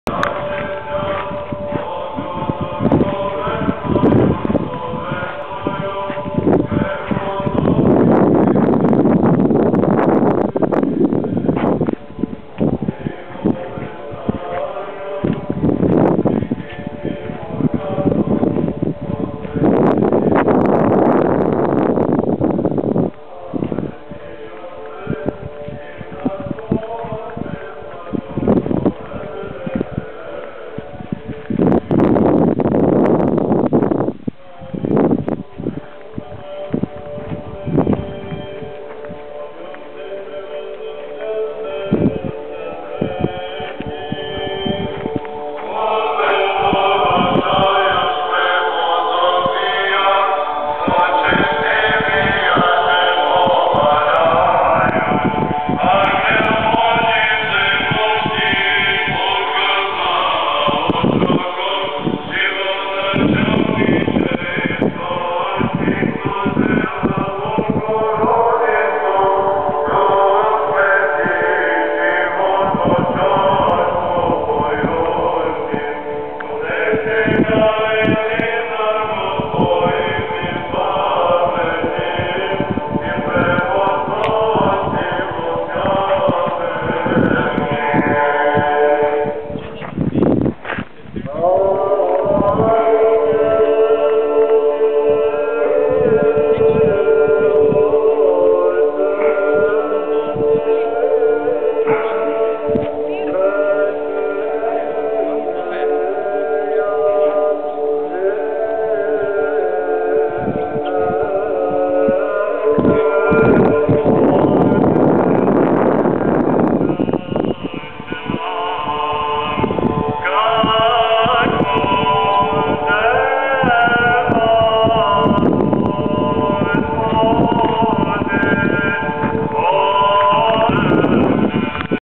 The Orthodox Church of St. Clement & St. Panteleimon at Plaosnik in Ohrid, Macedonia
It was built by the St. Clement in 863.